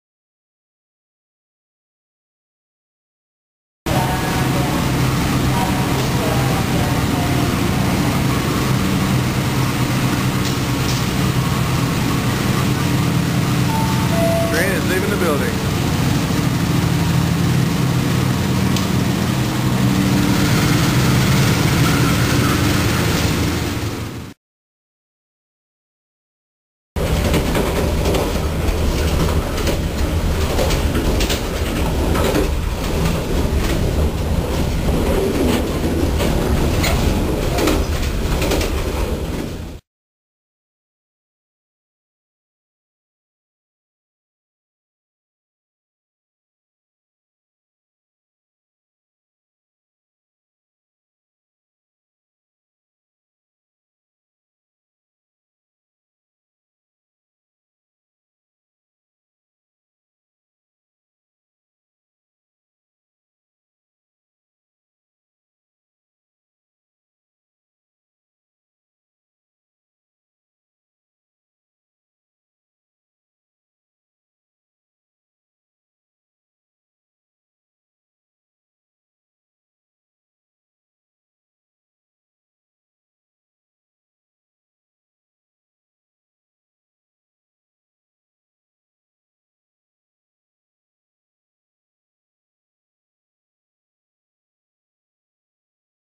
train to Yogya
Argo Willis trainride from Bandung to Yogya